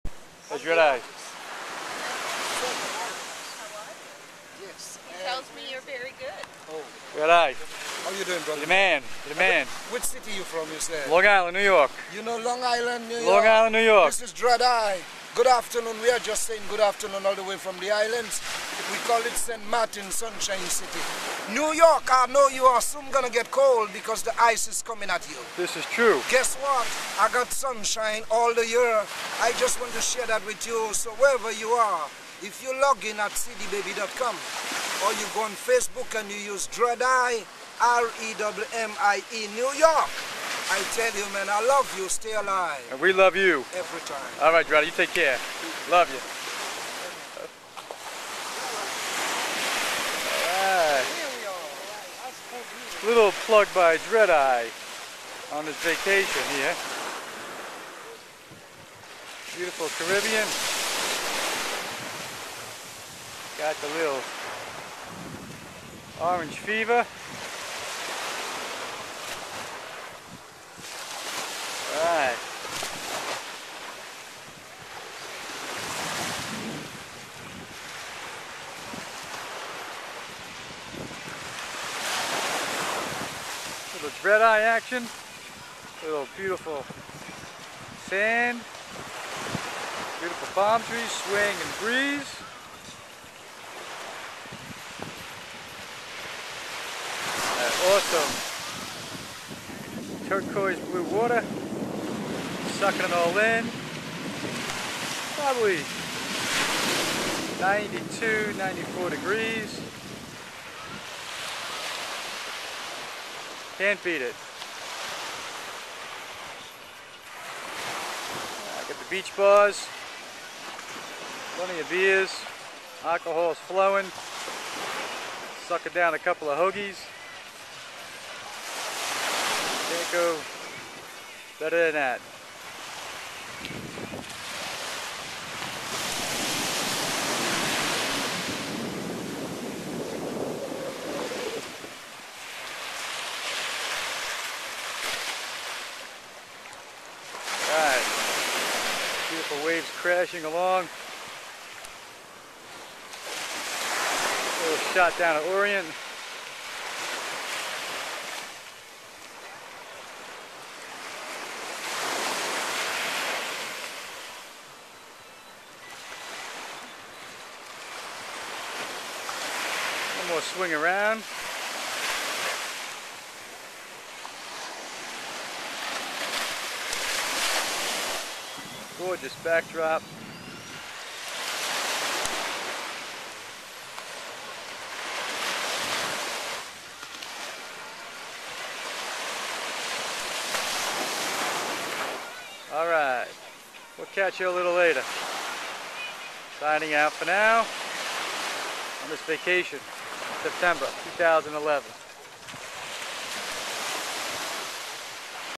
dreadi at orient beach, St. Martin.. 9-2011.MP4
Dreadi on orient beach, St. Martin. tropical storm Maria washed up a lot of seaweed on the beach.. usually Orient Beach is pristine clean white sand.. still as always, orient is the beach to be at when visiting St. Martin..